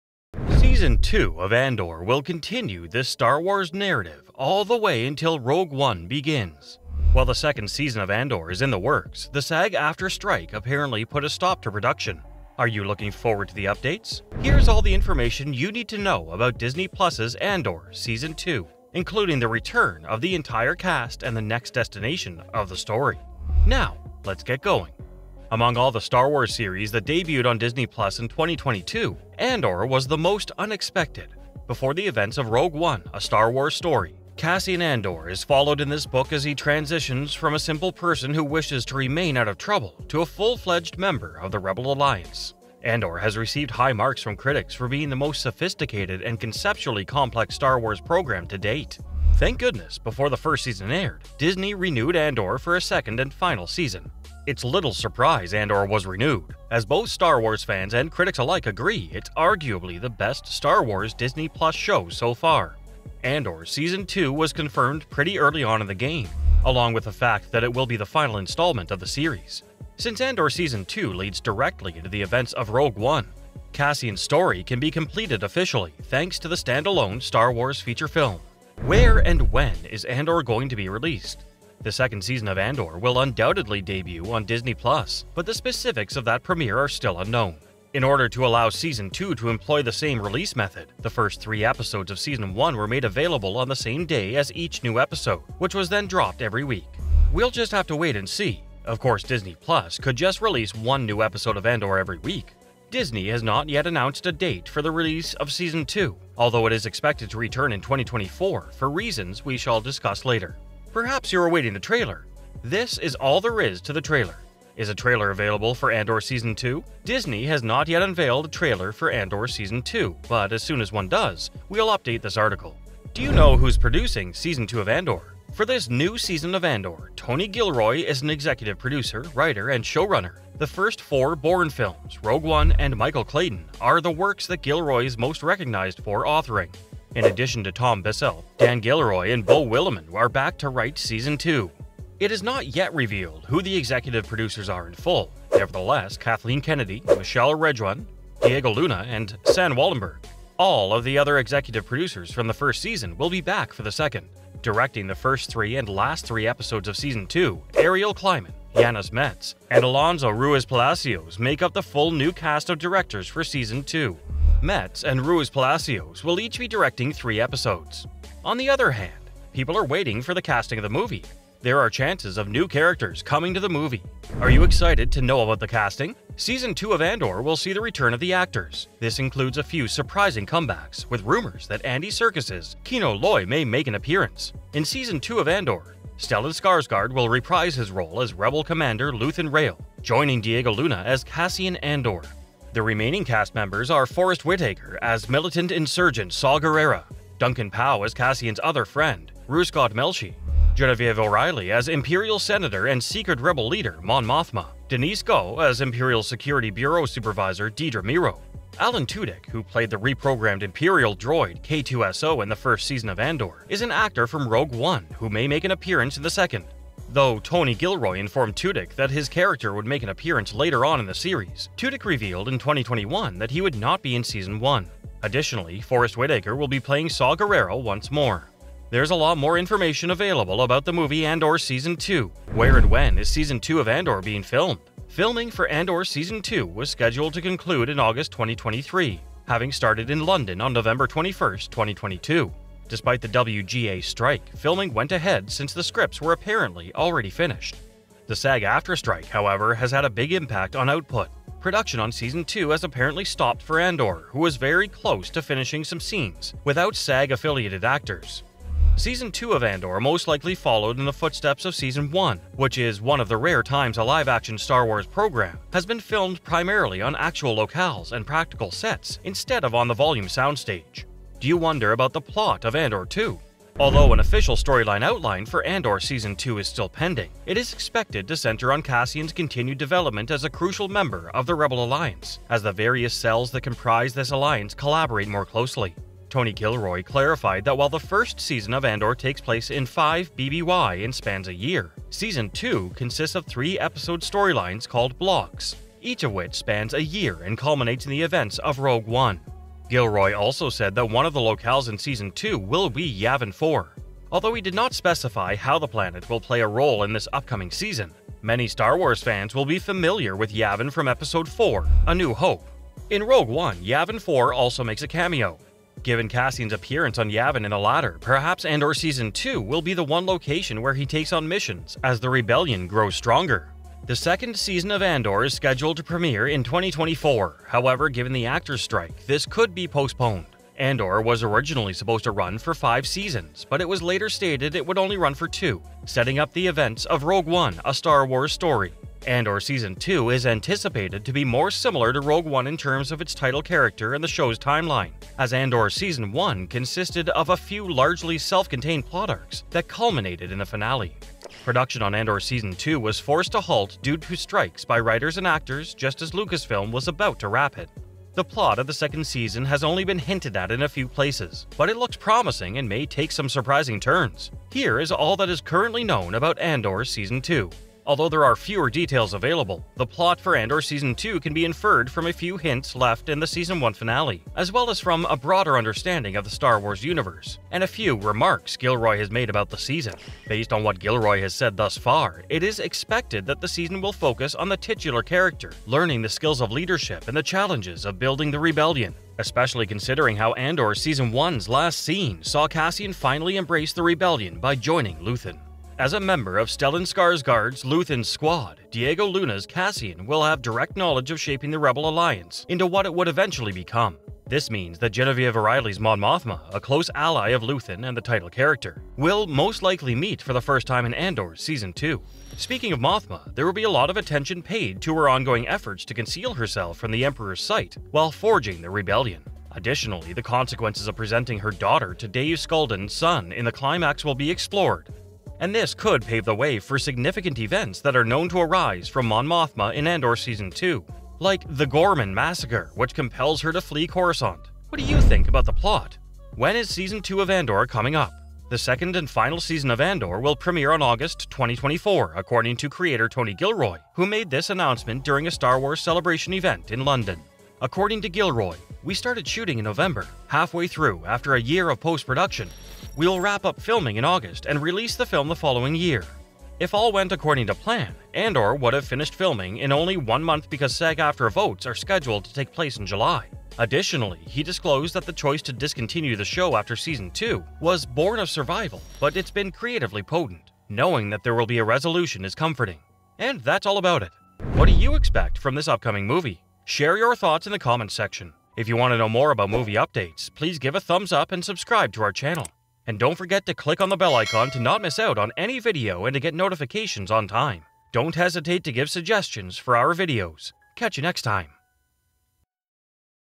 ANDOR SEASON 2 (2024): Everything We Need To Know So Far!
Welcome back to Binge-Watch Bliss, today on the channel we're going to be talking about Everything we know about Andor Season 2. If you're excited to learn more about this then make sure you watch the whole way through because you don't want to miss the details we have for you!